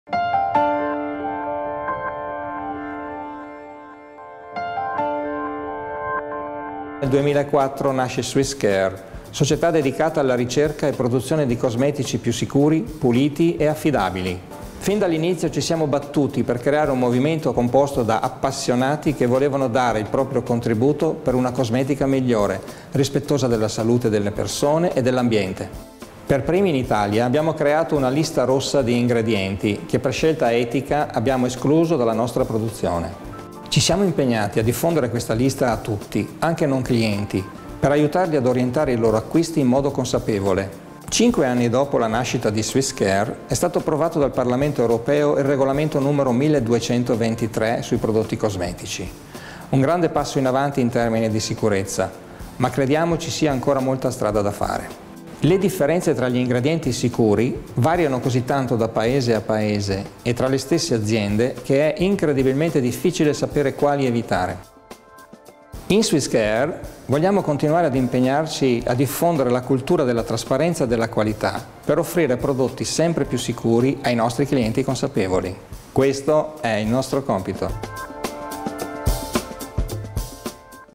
La Lista Rossa
Nel 2004 nasce SwissCare, società dedicata alla ricerca e produzione di cosmetici più sicuri, puliti e affidabili.

Fin dall’inizio ci siamo battuti per creare un movimento composto da appassionati che volevano dare il proprio contributo per una cosmetica migliore, rispettosa della salute delle persone e dell’ambiente.

Per primi in Italia abbiamo creato una lista «rossa» di ingredienti che, per scelta etica, abbiamo escluso dalla nostra produzione.
Ci siamo impegnati a diffondere questa lista a tutti, anche non clienti per aiutarli ad orientare i loro acquisti in modo «consapevole».

SwissCare vogliamo continuare ad impegnarci nel diffondere la cultura della trasparenza e della qualità per offrire prodotti sempre più sicuri per i nostri clienti consapevoli.

Questo è il nostro compito!